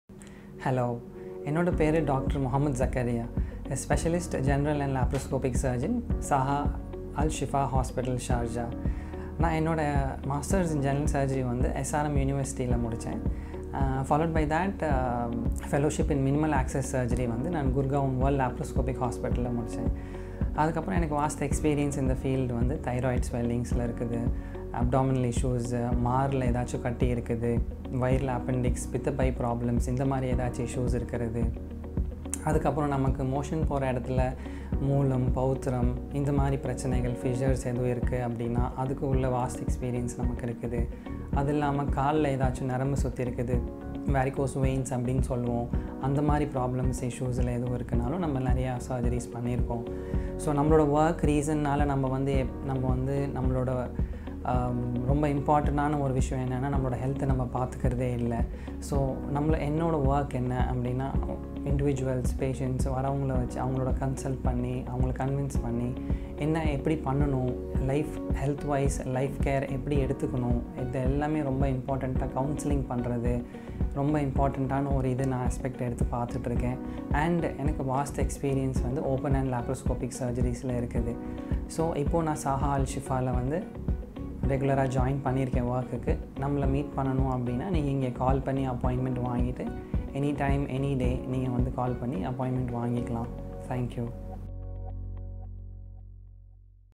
Laparoscopic Surgeon | General Surgeon |Dr. Muhammed Zakaria
With a vast experience in open and laparoscopic surgeries, Dr. Muhammad Zakaria, Specialist Laparoscopic and General Surgeon have expertise in conducting major and minor surgeries, like safe removal of the appendix, problems related to gall bladder, hernia, and piles. He is a fine addition to our surgical team.